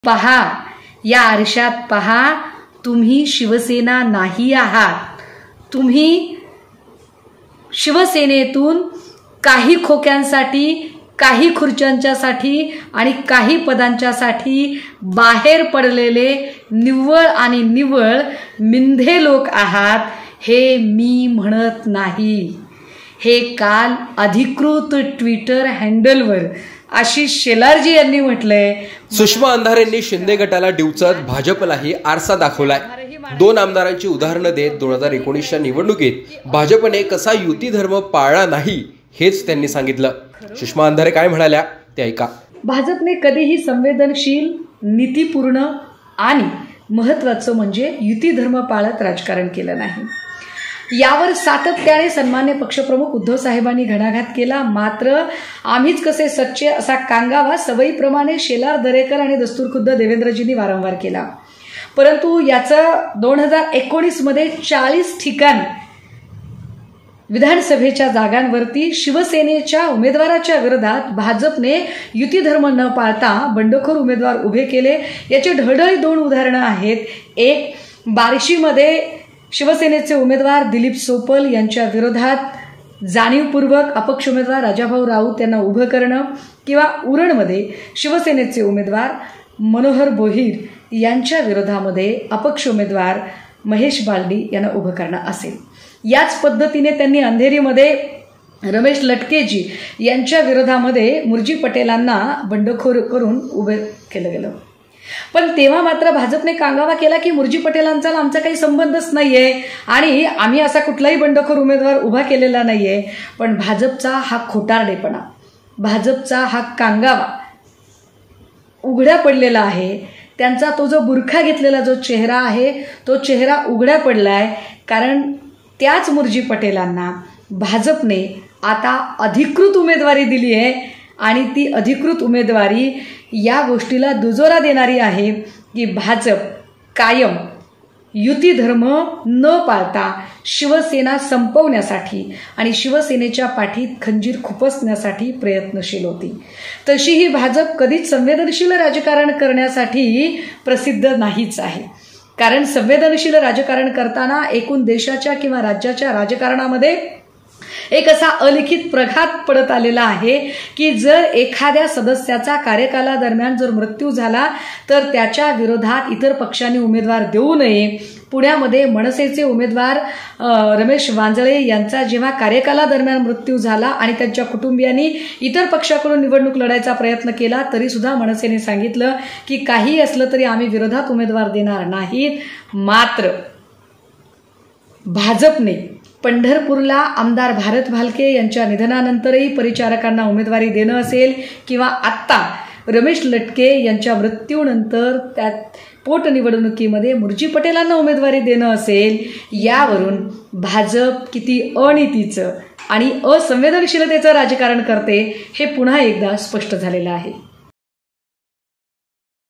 Sushma Andhare यांनी BJP च्या ४० जागांचा विषय काढला, दोन उदाहरणंही दिली... | Shiv Sena
सुषमा अंधारेंनी शिंदे गटाला डिवचत भाजपलाही आरसा दाखवला.. दोन आमदारांची उदाहरणं २०१९ च्या निवडणुकीत भाजपने कसा युती धर्म पाळला नाही.. हेच त्यांनी सांगितलं.. सुषमा अंधारे काय म्हणाल्या ऐका.. (Ashvin VO)